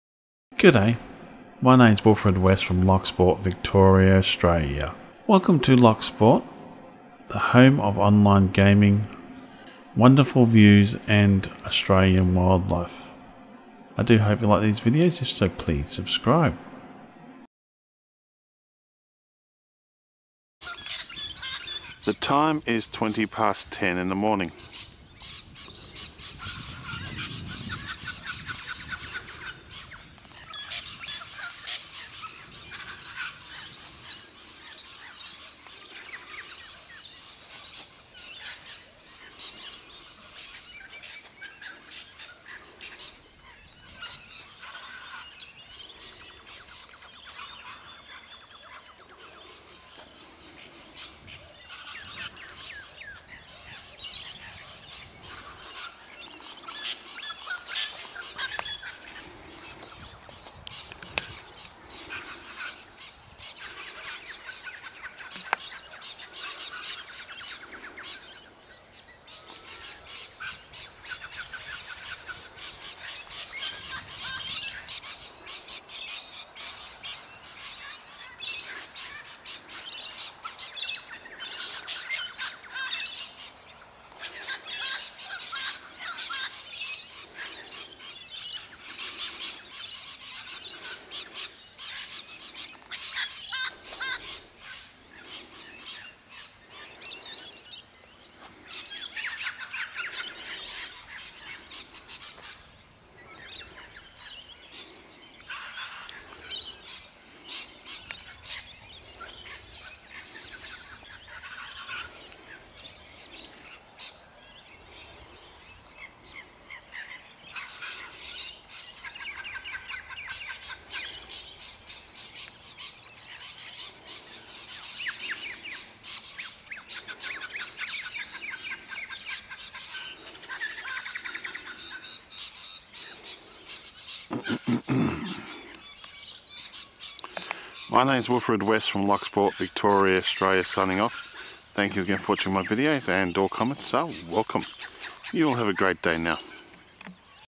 Looking at Lake Victoria on the 08-10-2017
This video was made with the Sony HXR-NX70U NXCAM Professional Camcorder with Auto Focus on the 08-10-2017 Res at 1920 x 1080 25 Fps and the voice over was made using the Sony's On-board Mic and the editing was done on my Hp Laptop with Adobe Premiere Pro CC 2017 and then Exported using Adobe Media Encoder CC 2017 with the res set at 1920 x 1080 with 50 Fps and the Size is 286 MB Length is 2:27 Video Type MP 4 and uploaded to YouTube with the res at 1920 x 1080 with 50 Fps.